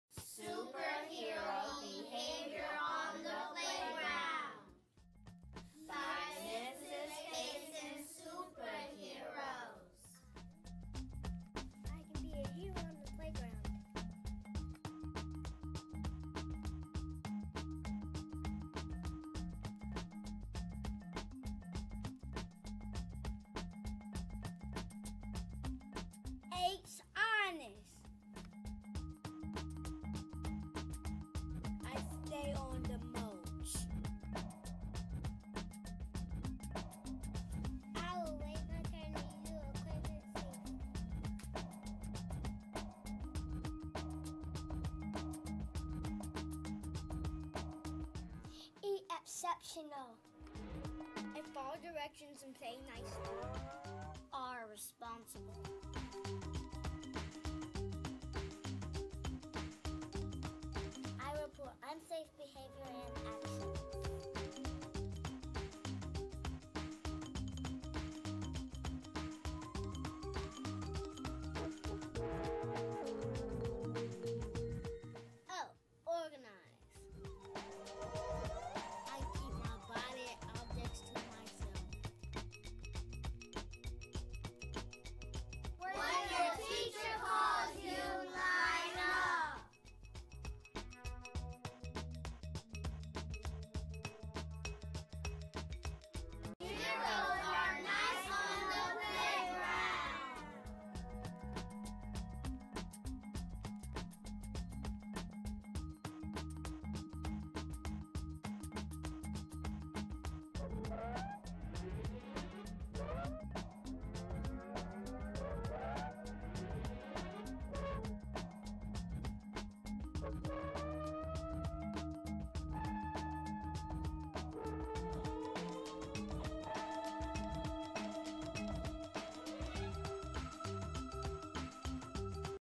Superhero Behavior in the Playground
Mrs. Faison's first graders created a movie to teach others how to do the right thing on the playground.